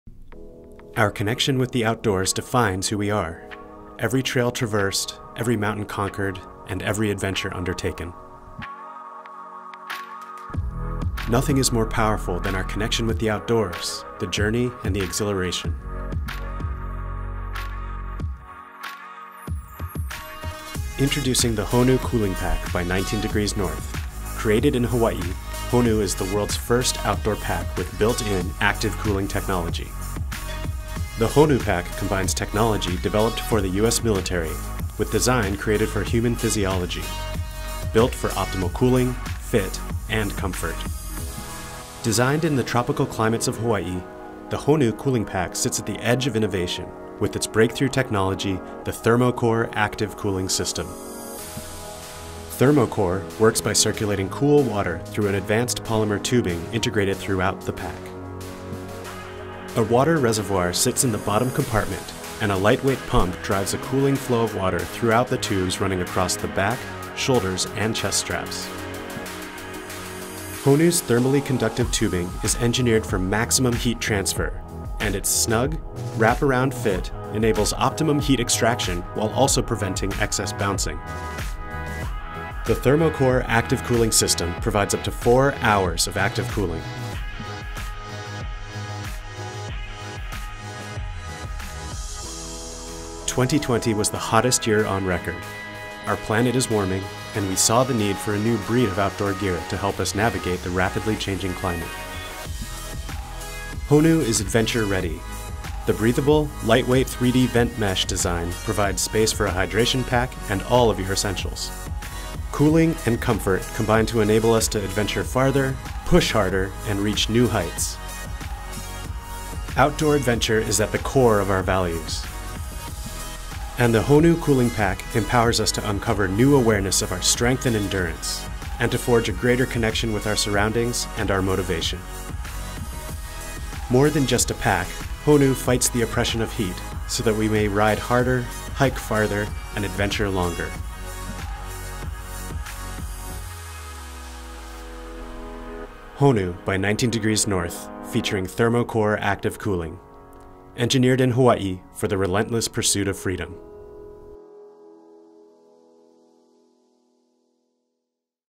Introducing the Honu cooling pack | Adventure-ready with ThermoCore Active Cooling Technology
19°N is on the leading edge of a new wave in climate enhancement equipment, the Honu Cooling Pack is the world’s first active cooling pack designed specifically for outdoor adventures.

ThermoCore® Active Cooling Technology is a network of advanced, thermally conductive microtubes embedded throughout the pack that cool your body. ThermoCore® provides up to 4 hours of active cooling. Years in the making, 19°N's extensive research, development, and military spec testing was conducted to develop a highly durable, thermally conductive tubing that is able to operate in extreme environments.

Made to support up to a half-day of high-exertion activity, the Honu’s active cooling system was tested and refined by feedback from professional triathletes and avid hikers in Hawaii's hottest and most demanding environments. In controlled laboratory testing, the Honu achieved twice the heat extraction of other wearable cooling products.